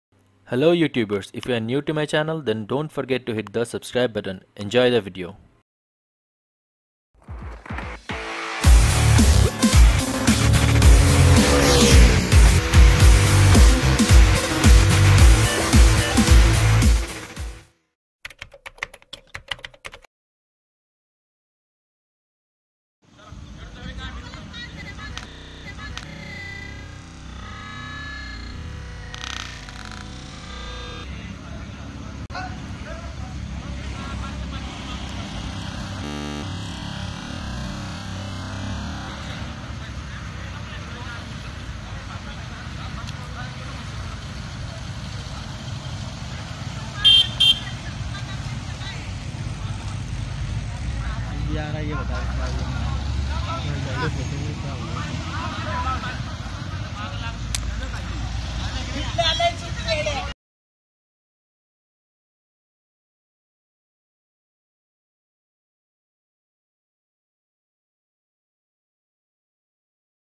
Violence Caught on Cam | Rage at Lavasa | Pune
Those were caught trying to sneak into the Lavasa city without paying the hefty entry fees of INR 200. Later on they were taken inside the city for some legal actions.